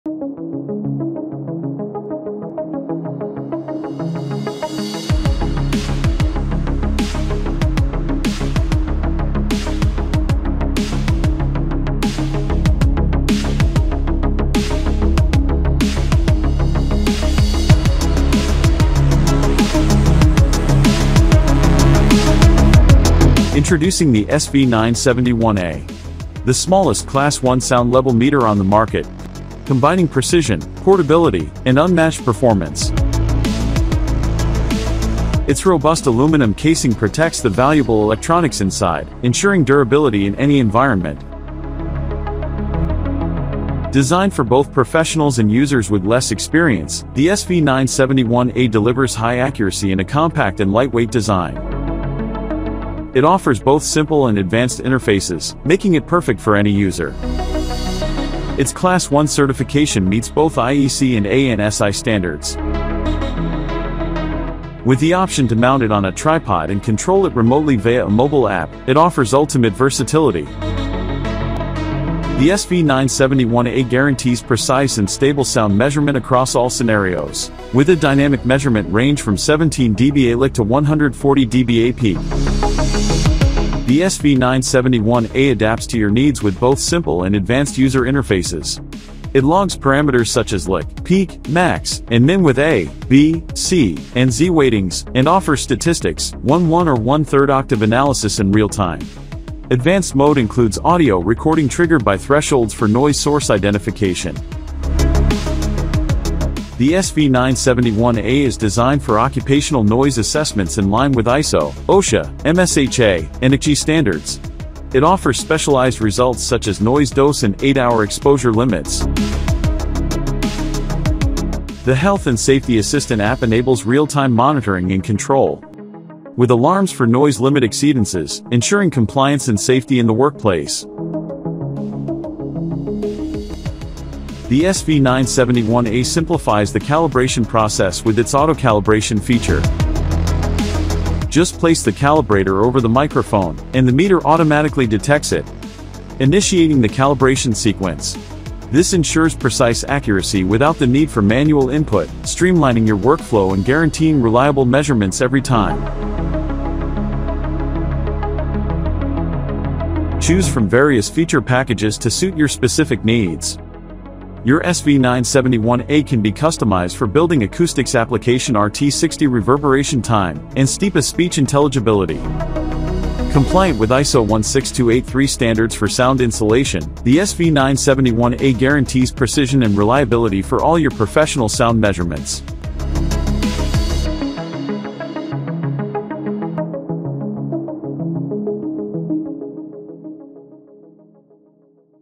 Class 1 Sound Level Meter | SV 971A | SVANTEK
The SV 971A is a Class 1 Sound Level Meter and Sound Exposure Meter designed for professional use, recognized for its compact size, lightweight, and inclusion of advanced features like a new microphone with a large dynamic range of 120 dB and low-power Bluetooth® connectivity for mobile application integration.

00:00 Intro
00:24 Overview
00:53 Interface
00:58 Standards
02:13 Calibration
02:38 Features & Options